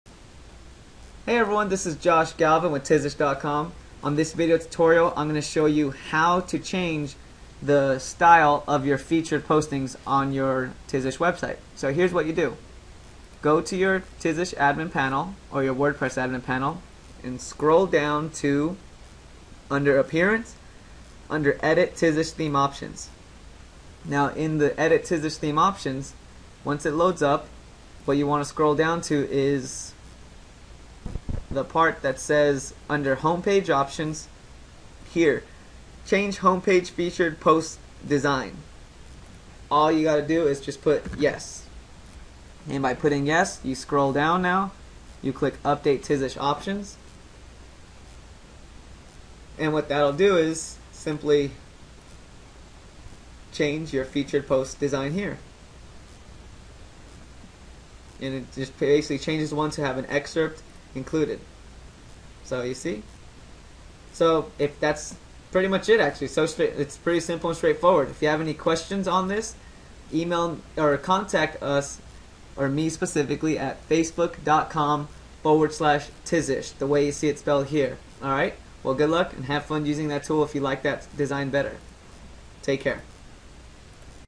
How to change your featured posts section style on your Tizish Wordpress blog
- In this video tutorial you will learn how to change the design of your featured posts section of your Tizish Wordpress Blog homepage. This is a quick change for those looking for some variations to their styling.